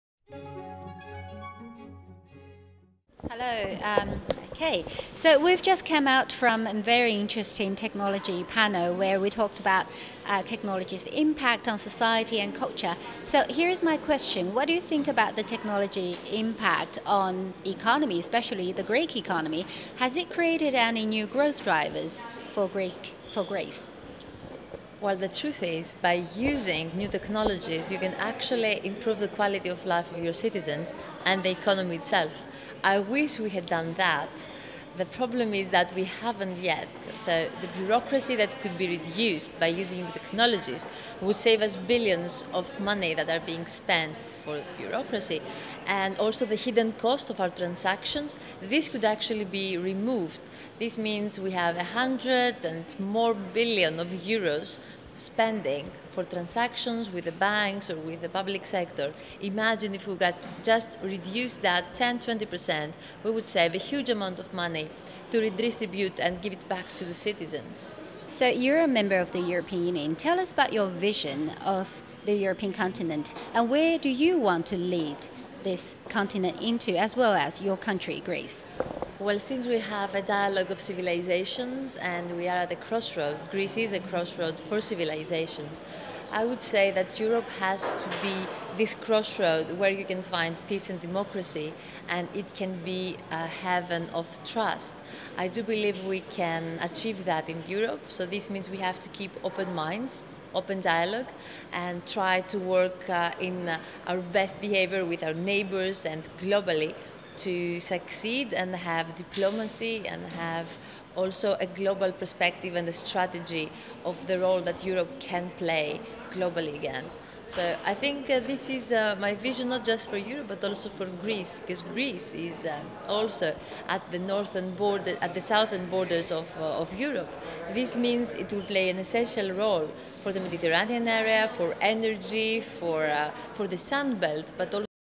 Eva Kaili talks technology and Europe
Greek MEP Eva Kaili talks about how technology can help create more growth drivers, and the need for Europe to cooperate to solve its problems